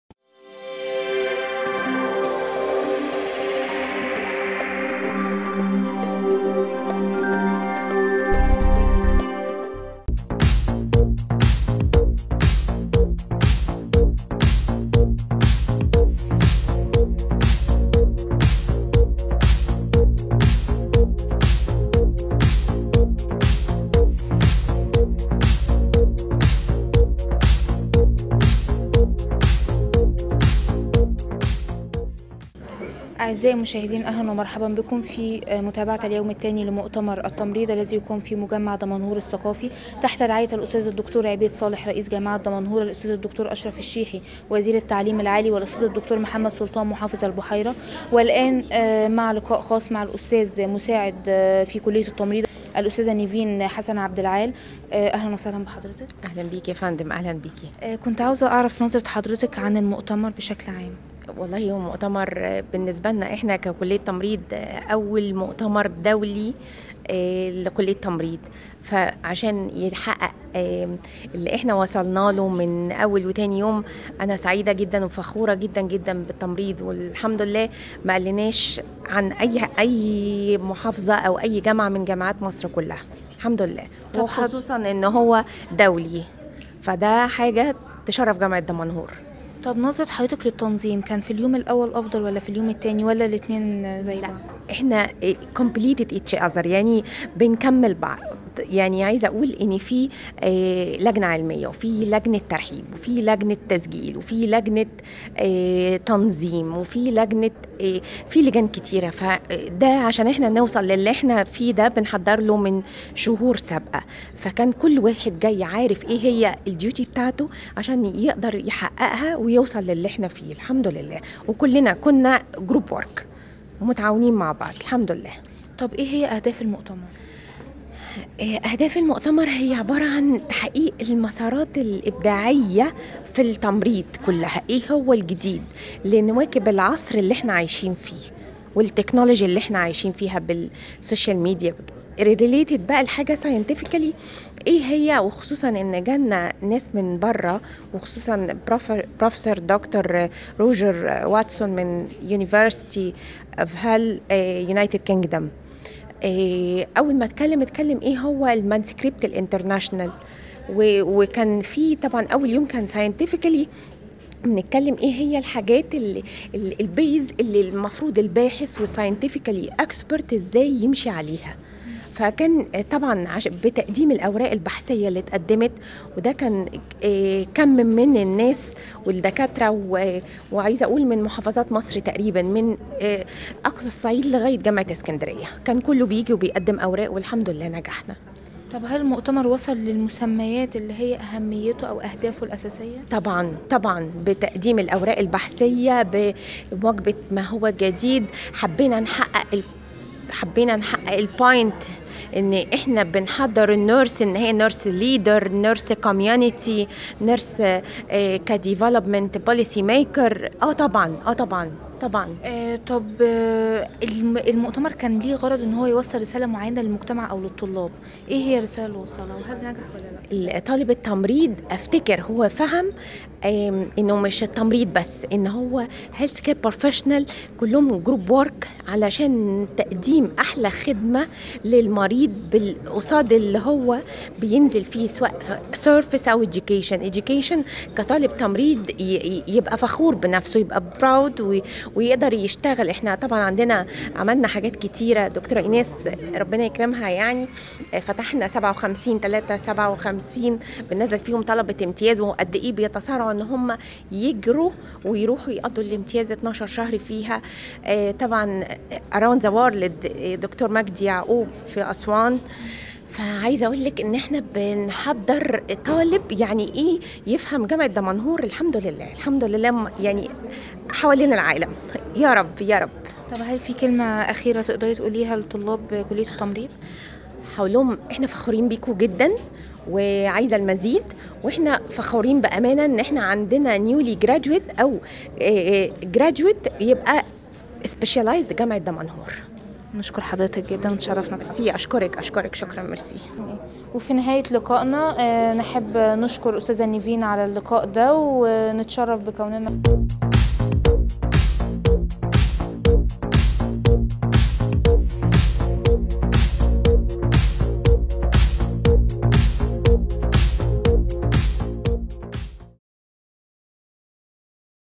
عايدة الوكيل .. وحوار تليفزيوني مع الأستاذة "نيفين حسن عبدالعال" ll قناة جامعة دمنهور ll
ll قناة جامعة دمنهور ll
عايدة الوكيل .. "فريق الدعم الإعلامي بجامعة دمنهور"
و حوار تليفزيوني مع الأستاذة " نيفين حسن عبدالعال "
استاذ مساعد بكلية التمريض جامعة دمنهور
علي هامش المؤتمر الدولي الأول للتمريض
تحت عنوان " المسارات الإبداعية للتمريض في العصر الحديث "